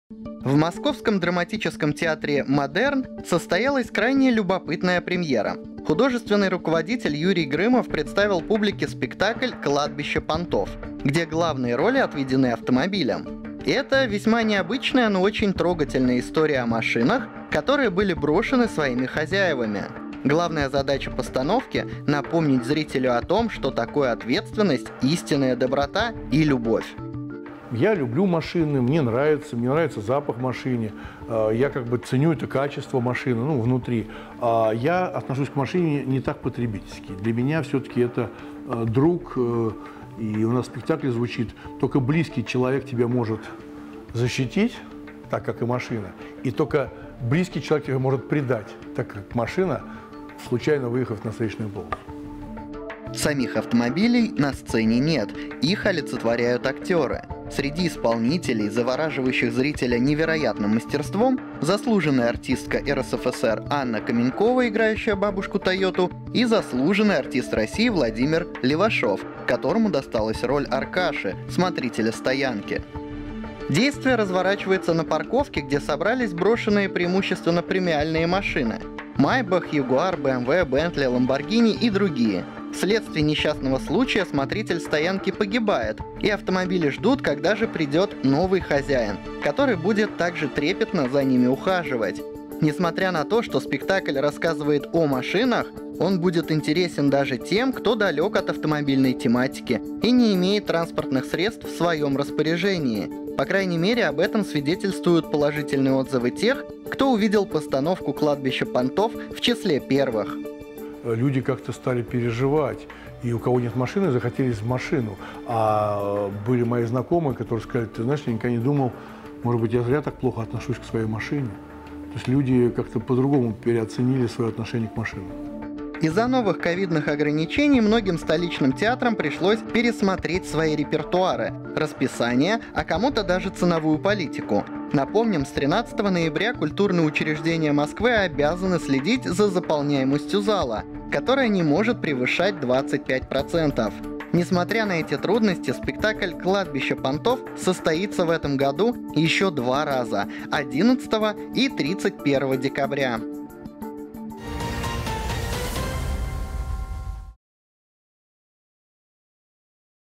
В Москве прошла премьера спектакля про автомобили | Новости с колёс №1253
В московском драматическом театре «Модерн» состоялась любопытная премьера. Художественный руководитель Юрий Грымов представил публике спектакль «Кладбище понтов», где главные роли отведены автомобилям. Это — весьма необычная, но очень трогательная история о машинах, которые были брошены своими хозяевами.